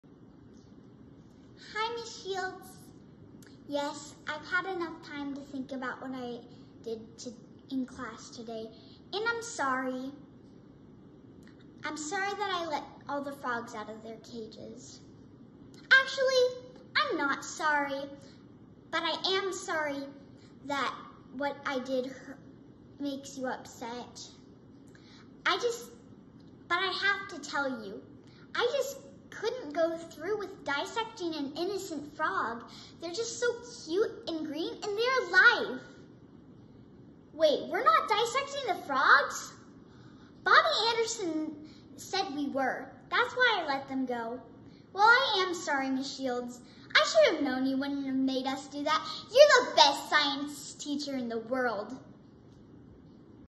Very new monologue I just started to learn!
I am posting this so I can be considered for the new monologue slam and so when I improve this with lots and lots of practice I can look back on this and see my improvements.